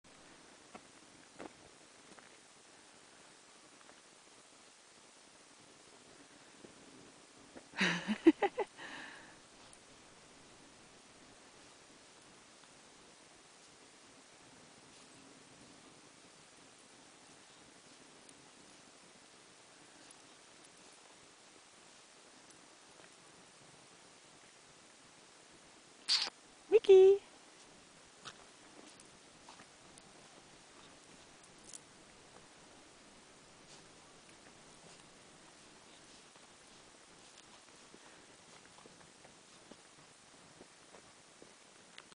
Eva Pictures presents: Micky of the snowy racketeers
Dieser FIlm von Eva Lindemann zeigt wunderschön, dass ihre "Knotterbacke" in Wirklichkeit ein ganz süsser Bub ist .